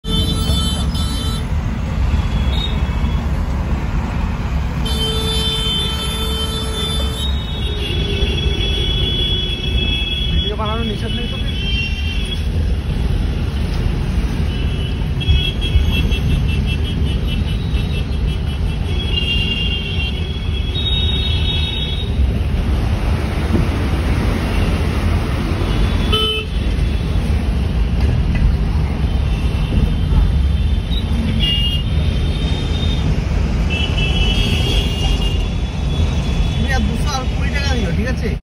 Kolkata Howrah bridge in West Bengal
Ajj hum Gaye the Howrah bridge dekhne Kolkata main bohut hi jyada khubsurat hai yr 😮